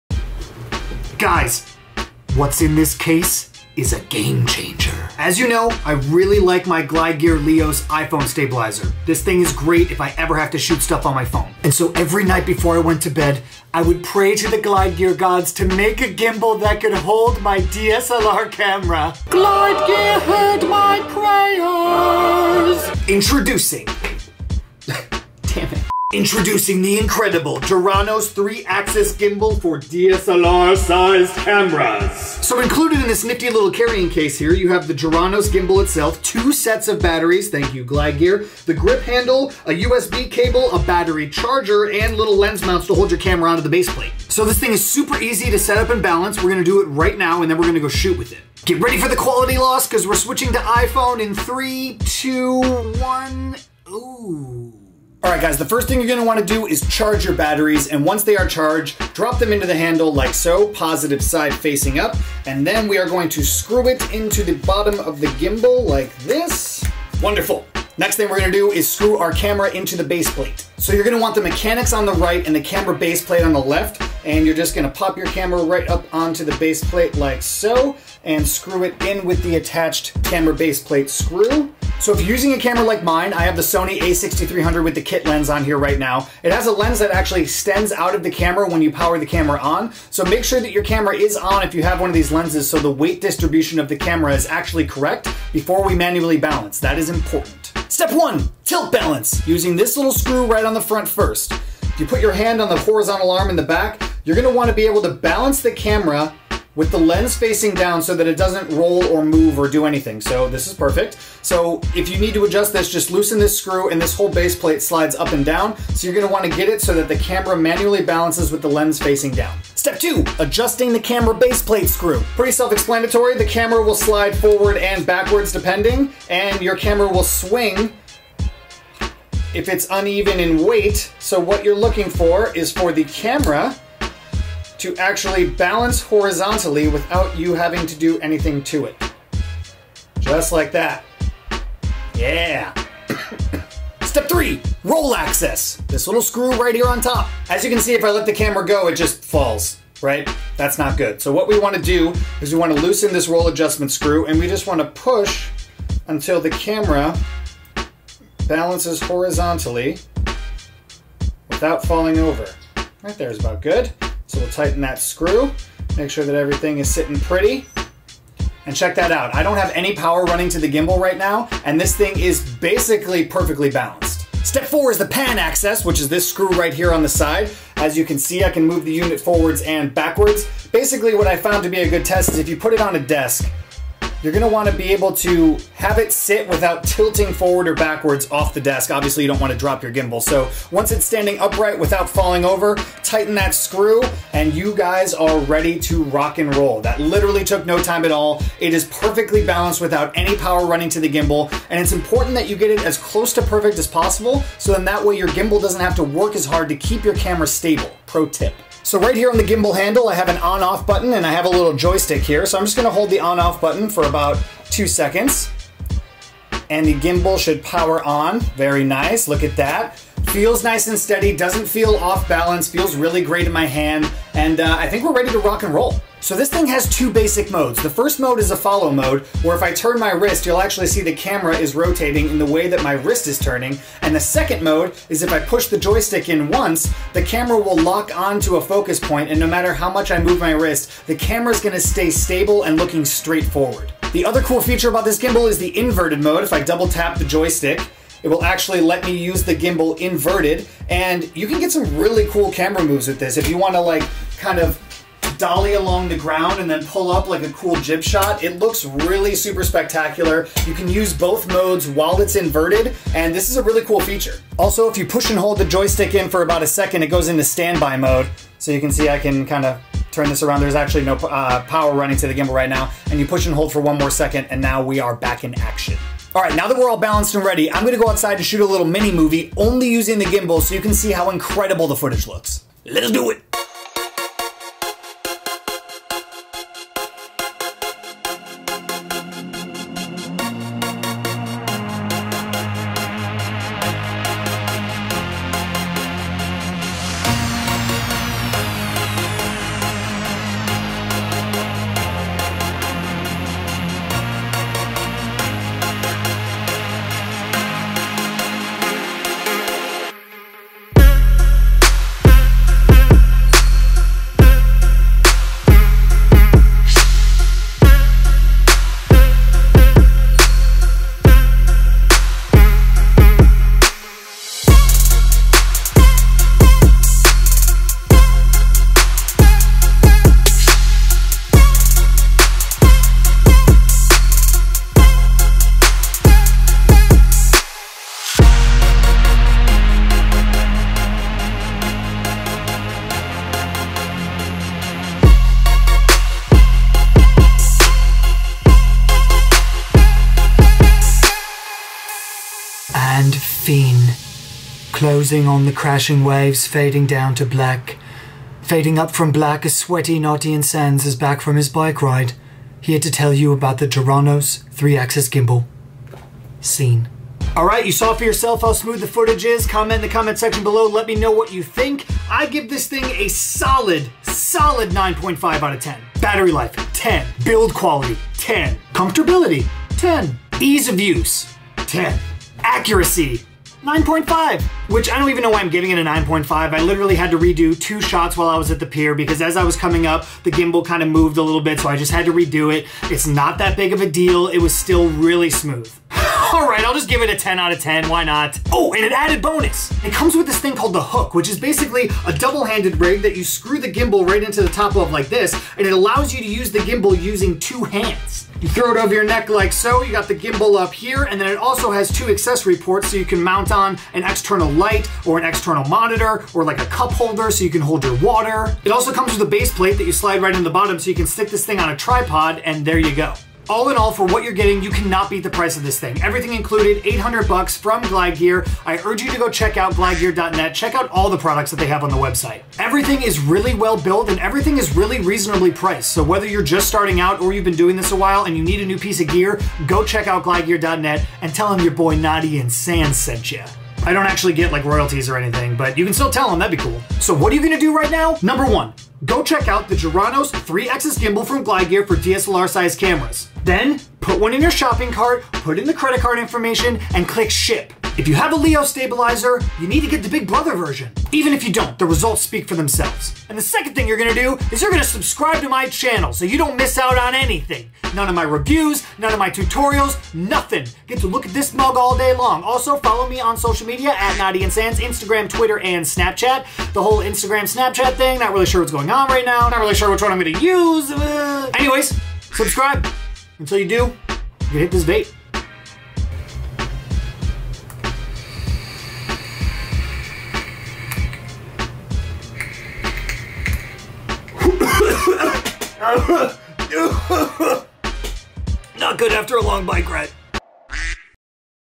Glide Gear Geranos 3-Axis Gimbal Review
In this video, we take a look at the BALLER Glide Gear Geranos 3-Axis Gimbal for DSLR Cameras. Learn how to properly balance it, and take a look at some test footage!